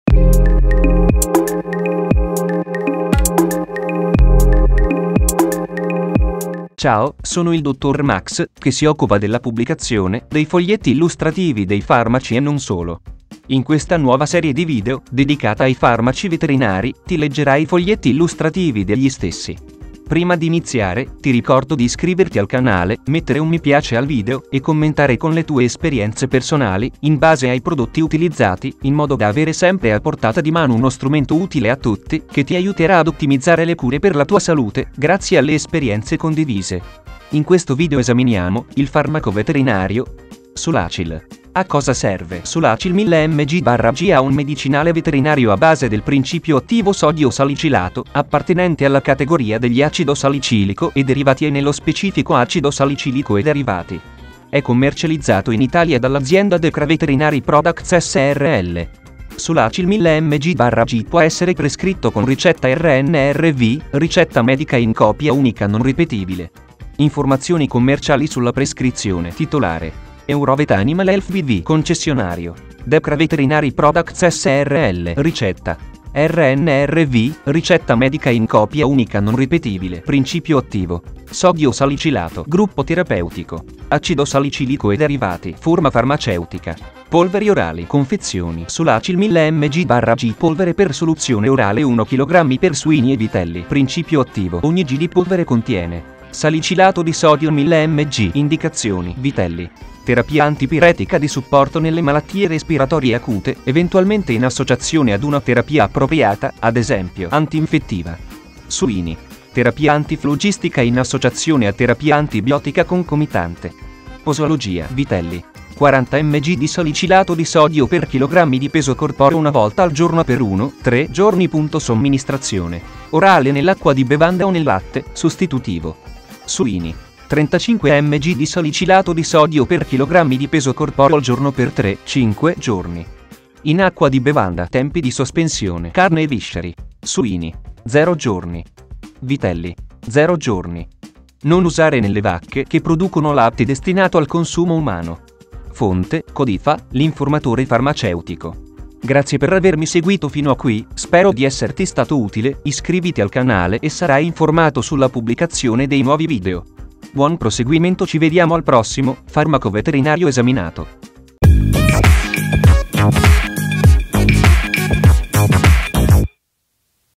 🐶 SOLACYL Farmaco Veterinario a cosa serve ? - Foglietto Illustrativo Salute
Abbiamo letto il Foglietto Illustrativo e parlato del Farmaco Veterinario: SOLACYL

Capitoli ⏱
0:00 Introduzione
0:06 Presentazione
0:40 Farmaco Veterinario KIBIS
1:01 A cosa serve ?

SOLACYL Farmaco Veterinario